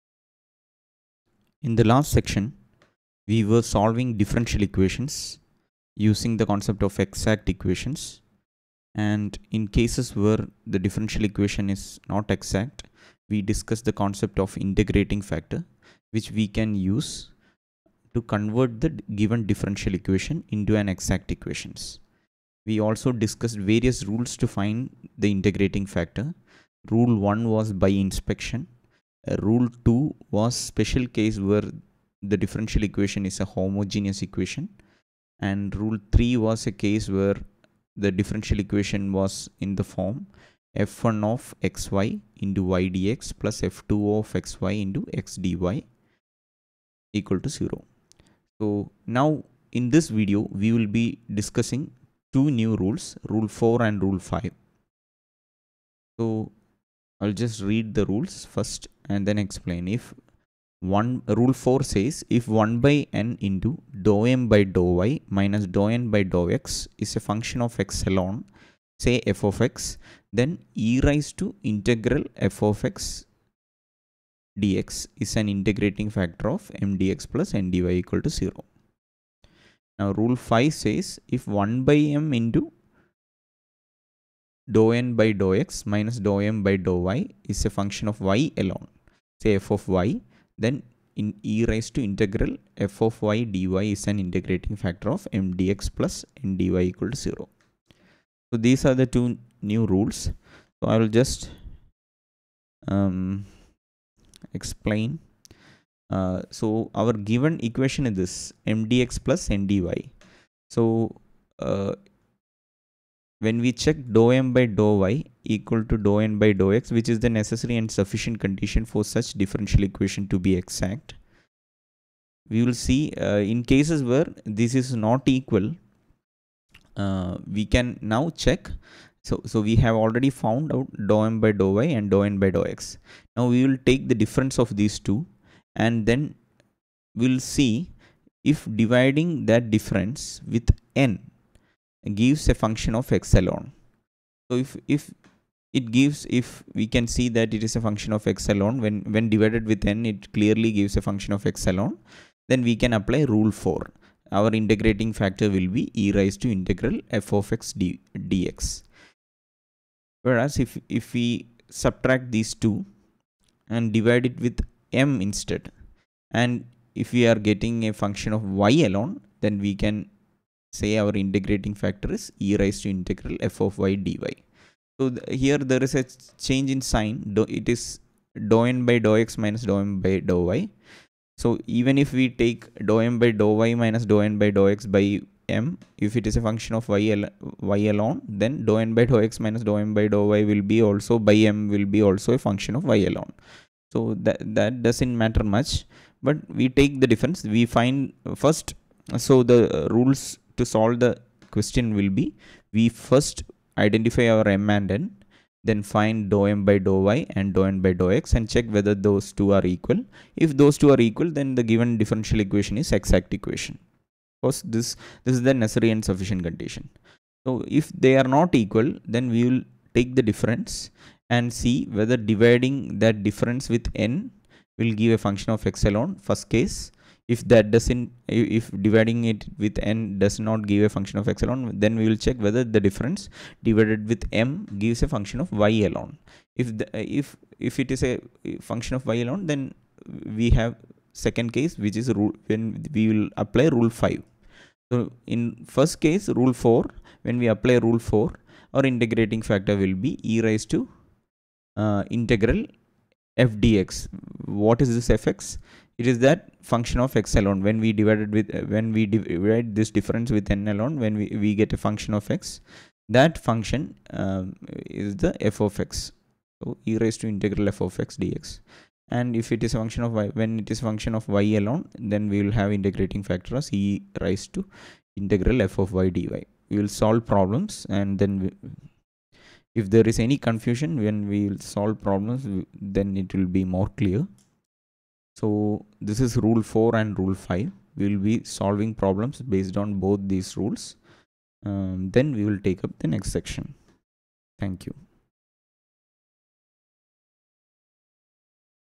Solving DE ; Integrating Factor ; Rule IV : 1/N(∂M/∂y-∂N/∂x) is a function of x alone ; Rule V ...
Solving Exact Differential Equations
Integrating Factor
 Rule IV : if 1/N(∂M/∂y-∂N/∂x) is a function of x alone, say f(x), Integrating Factor (IF) = e^(∫ f(x)dx)

Rule V : if 1/M(∂N/∂x-∂M/∂y) is a function of y alone, say f(y), Integrating Factor (IF) = e^(∫ f(y)dy)